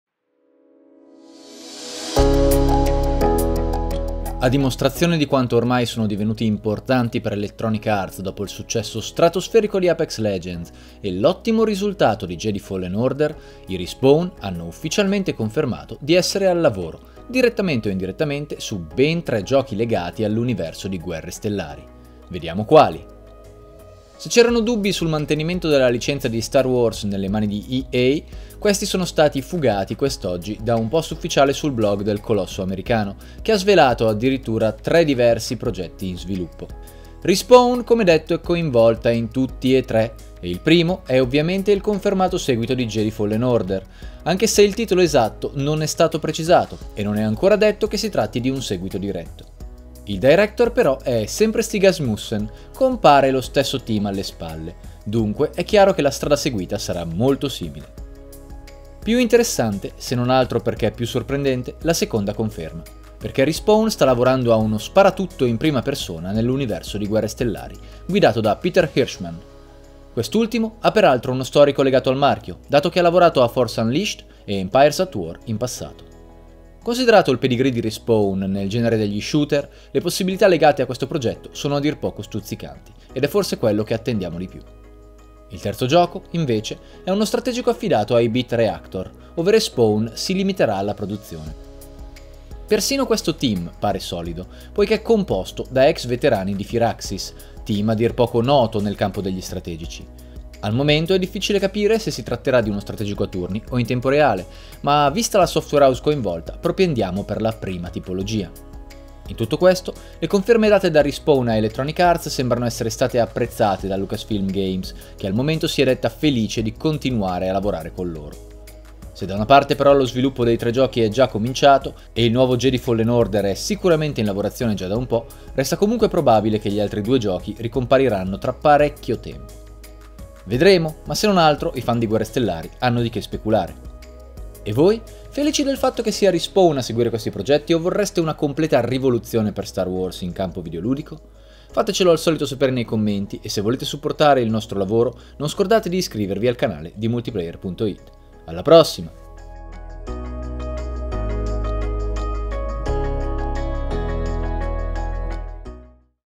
Annunciati 3 Nuovi Giochi di STAR WARS!
È ufficiale: Electronic Arts e Lucasfilm Games hanno annunciato che Respawn Entertainment (Apex Legends, Titanfall, Star Wars Jedi: Fallen Order) sta sviluppando tre nuovi giochi basati su Star Wars: Il seguito di Star Wars Jedi: Fallen Order, un nuovo gioco sparatutto in prima persona a tema Star Wars e un gioco di strategia.